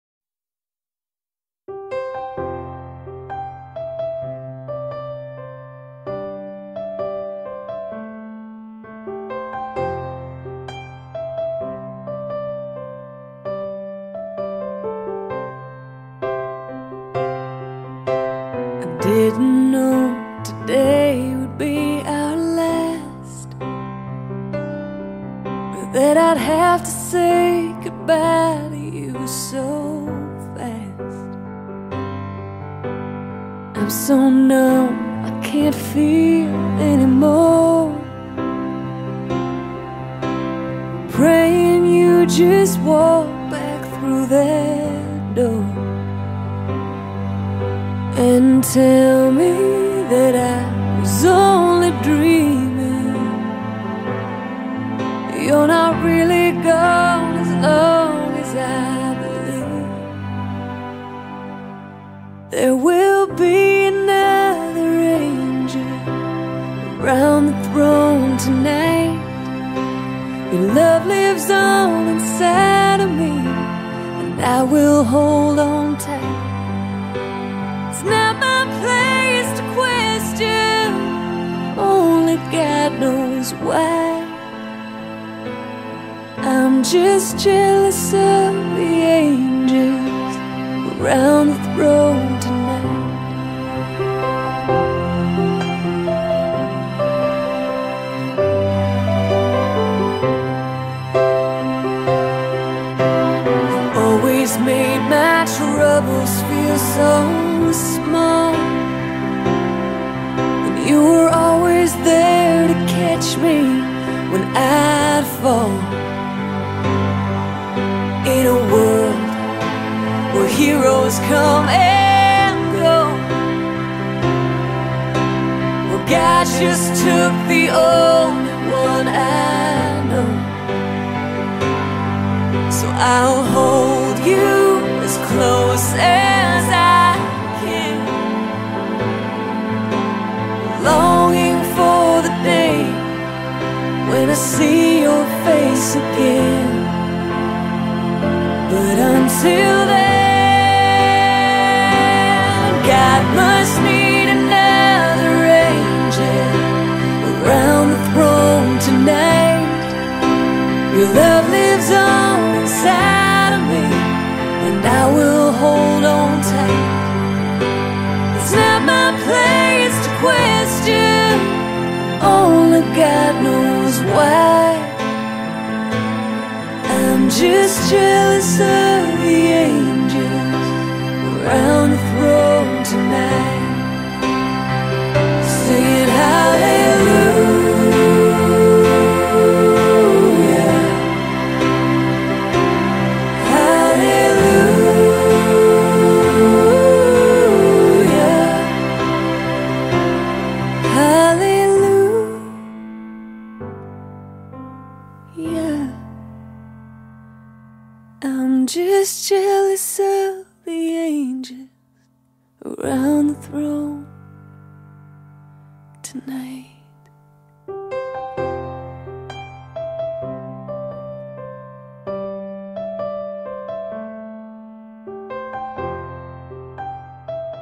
Paul Walker Tribute
This song has been stuck in my head since his passing so I made a slide show of Paul with his fast family. He will forever be loved and missed.